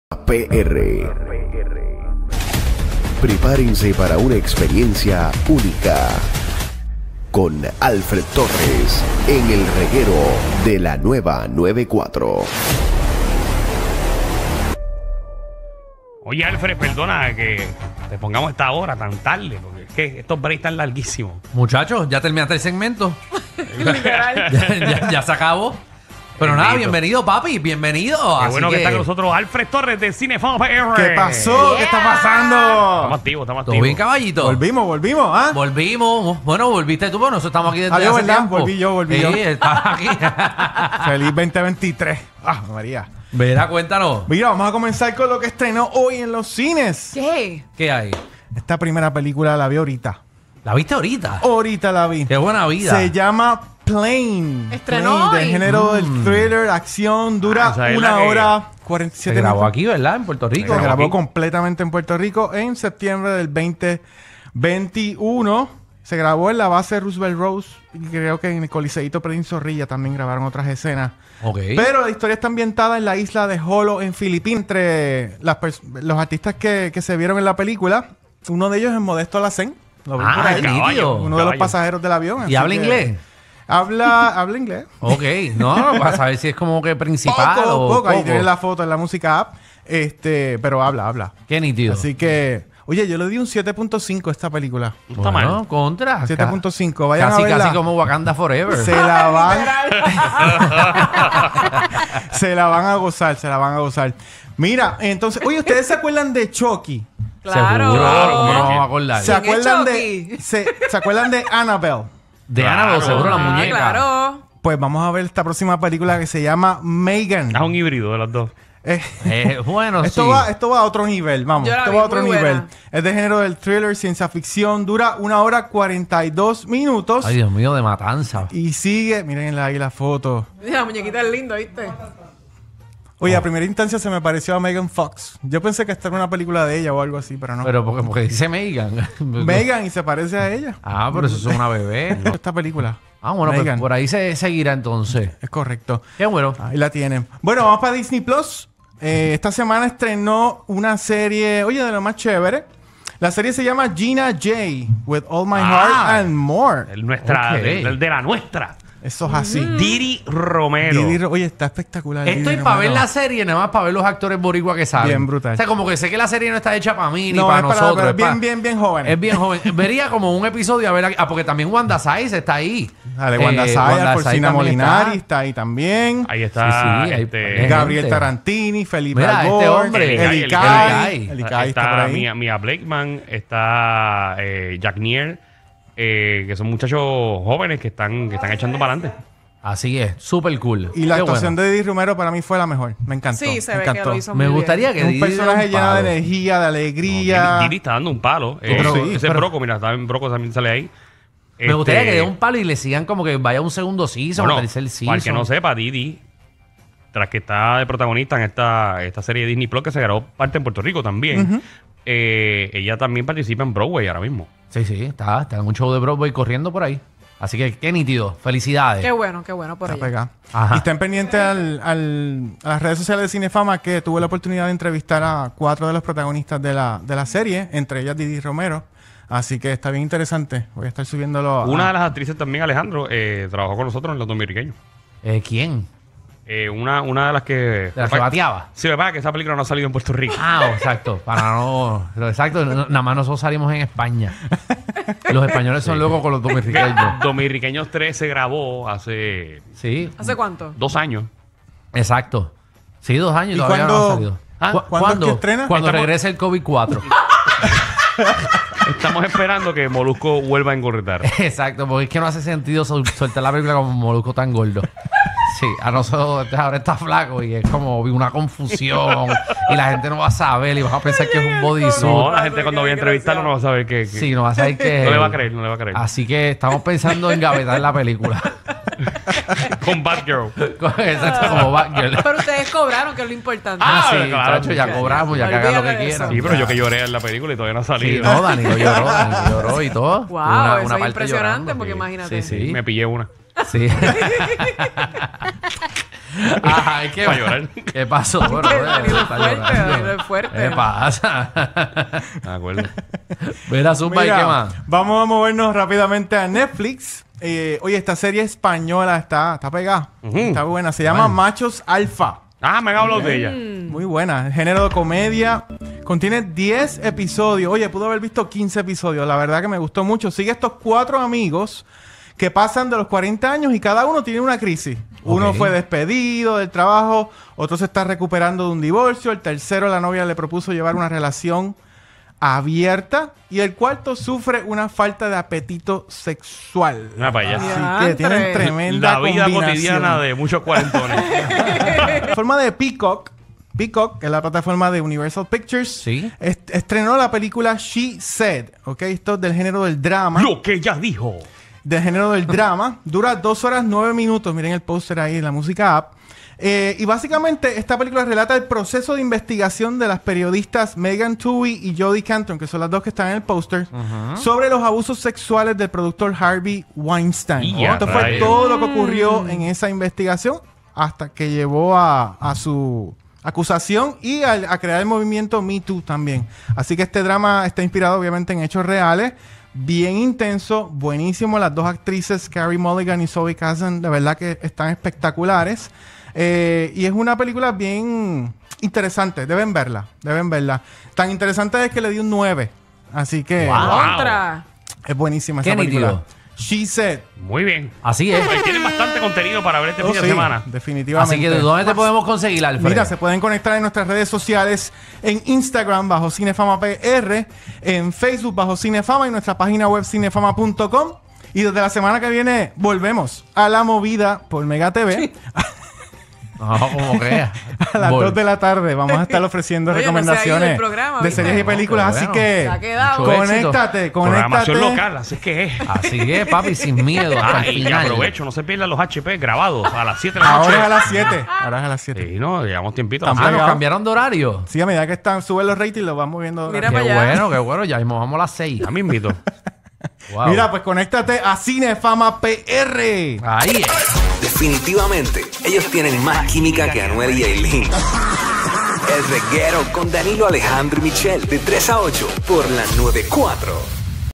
CINE FAMA ElReguero Enero 12, 2023 ✅
Entérate con Alfred de lo más reciente en la industria del entretenimiento y estrenos en cines y plataformas de streaming.